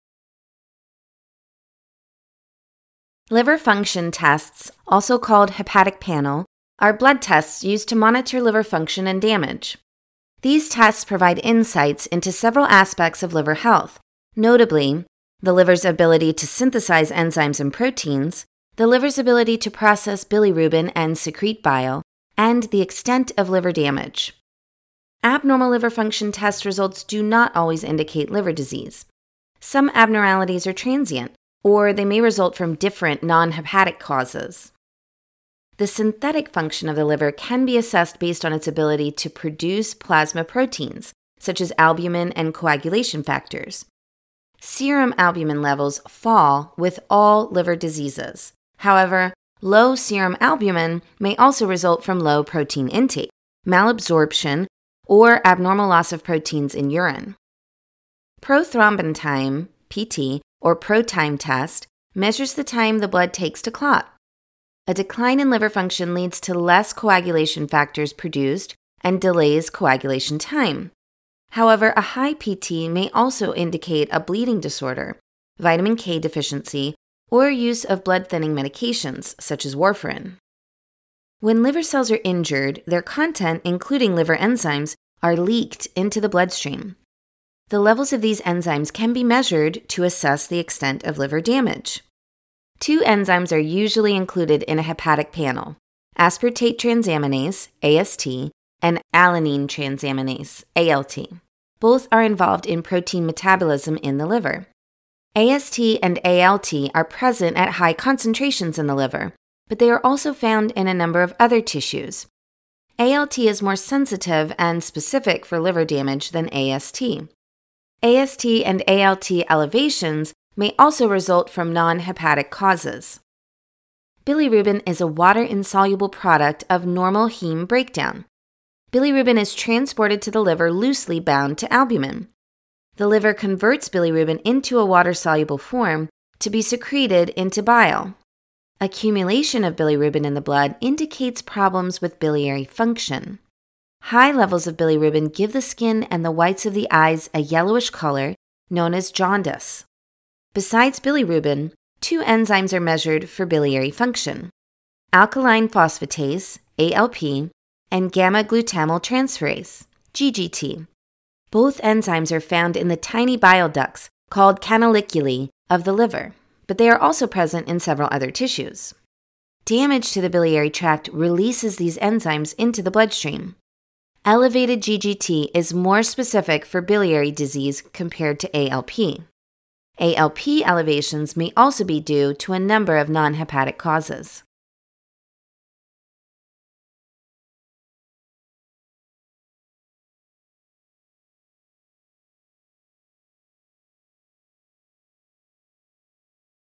Liver Function Tests (LFTs), Animation
(USMLE topics) Liver labs: AST, ALT, bilirubin, albumin, prothrombin time, ALP, and GGT.

Liver function tests, also called hepatic panel, are blood tests used to monitor liver function and damage. These tests provide insights into several aspects of liver health, notably:
- the liver’s ability to synthesize enzymes and proteins,
- the liver’s ability to process bilirubin and secrete bile,
- and the extent of liver damage.
Abnormal liver function test results do not always indicate liver disease. Some abnormalities are transient; or they may result from different, non-hepatic causes.
The synthetic function of the liver can be assessed based on its ability to produce plasma proteins, such as albumin and coagulation factors. Serum albumin levels FALL with all liver diseases. However, low serum albumin may also result from low protein intake, malabsorption or abnormal loss of proteins in urine.
Prothrombin time, PT, or pro time test, measures the time the blood takes to clot. A decline in liver function leads to less coagulation factors produced and delays coagulation time. However, a high PT may also indicate a bleeding disorder, vitamin K deficiency, or use of blood-thinning medications, such as warfarin.
When liver cells are injured, their content, including liver enzymes, are leaked into the bloodstream. The levels of these enzymes can be measured to assess the extent of liver damage. Two enzymes are usually included in a hepatic panel: aspartate transaminase, AST, and alanine transaminase, ALT; both are involved in protein metabolism in the liver. AST and ALT are present at high concentrations in the liver, but they are also found in a number of other tissues. ALT is more sensitive and specific for liver damage than AST. AST and ALT elevations may also result from non-hepatic causes.
Bilirubin is a water-insoluble product of normal heme breakdown. Bilirubin is transported to the liver loosely bound to albumin. The liver converts bilirubin into a water-soluble form to be secreted into bile. Accumulation of bilirubin in the blood indicates problems with biliary function. High levels of bilirubin give the skin and the whites of the eyes a yellowish color known as jaundice.
Besides bilirubin, two enzymes are measured for biliary function: alkaline phosphatase, ALP, and gamma-glutamyltransferase, GGT. Both enzymes are found in the tiny bile ducts, called canaliculi, of the liver; but they are also present in several other tissues. Damage to the biliary tract releases these enzymes into the bloodstream. Elevated GGT is more specific for biliary disease compared to ALP.  ALP elevations may also be due to a number of non-hepatic causes.